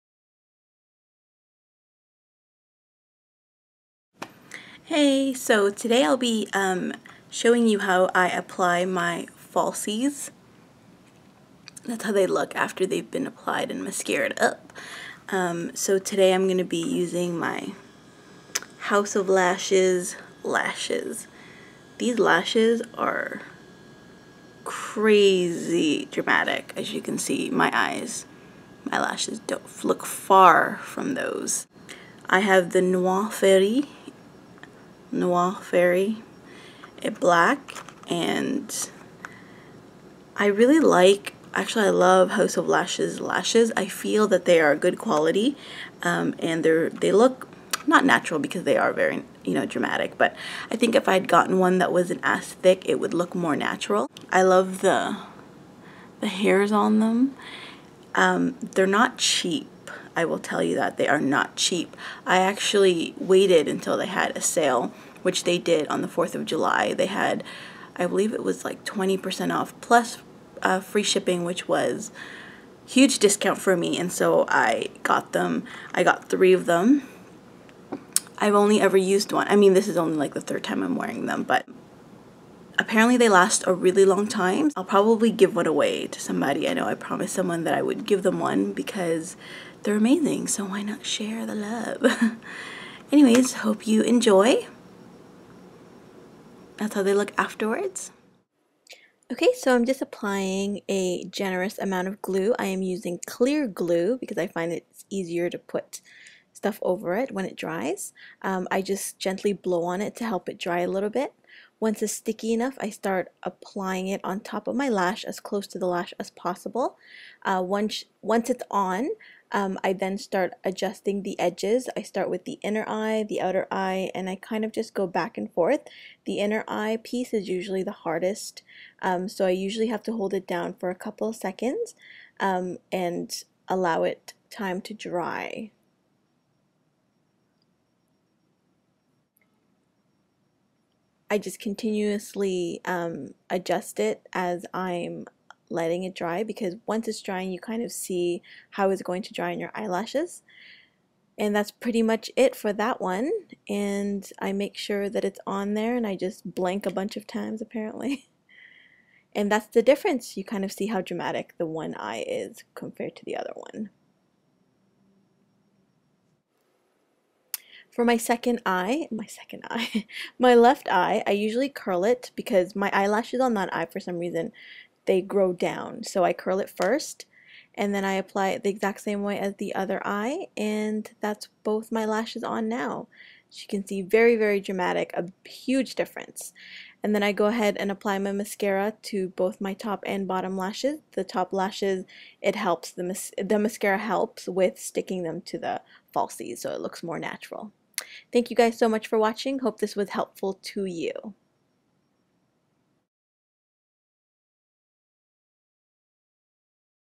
Falsies Tutorial | House of Lashes Review
I have recently started using House of Lashes, lashes, and I absolutely LOVE them! I wanted to do a review and figured, why not just throw in how I apply them as well. Enjoy!

I am in no way associated with House of Lashes. I am simply a consumer who made a purchase.